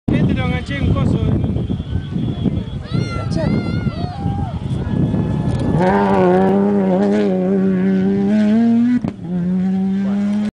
Dakar 2011 - Nissan Lavielle y Polato
Un Nissan Proto llegando a Baradero en la final del Rally Dakar 2011 Argentina Chile, tercera edición en Sudamérica.